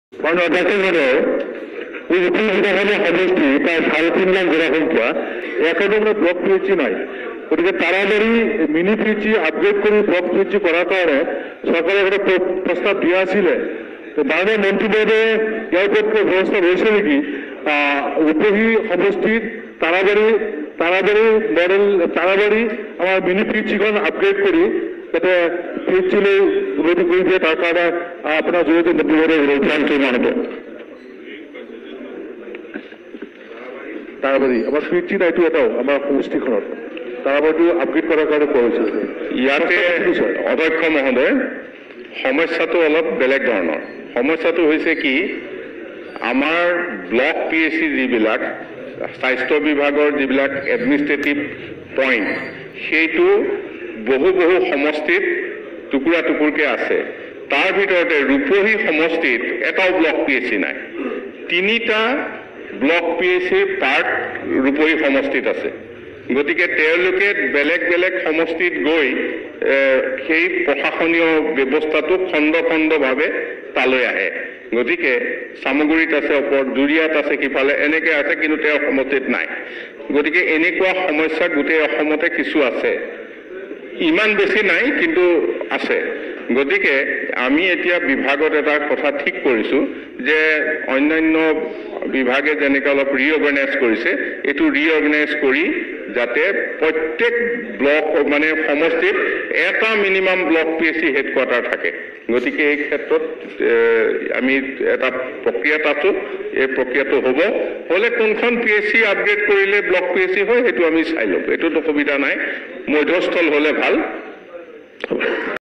বিধানসভাৰ মজিয়াত বিধায়ক নুৰুল হুদাৰ প্ৰশ্ন/মন্ত্ৰী কেশৱ মহন্তৰ উত্তৰ/তাৰাবাৰী ব্লক হস্পিটেল
অসম বিধানসভাৰ মজিয়াত বিধায়ক নুৰুল হুদাই প্ৰশ্ন উত্থাপন কৰে#
তাৰাবাৰী মিনি প্ৰাথমিক স্বাস্থ্য কেন্দ্ৰক ব্লক স্বাস্থ্য কেন্দ্ৰলৈ উন্নীতকৰণৰ দাবী জনায়#
মন্ত্ৰী কেশৱ মহন্তই ইতিবাচক সঁহাৰি জনায়#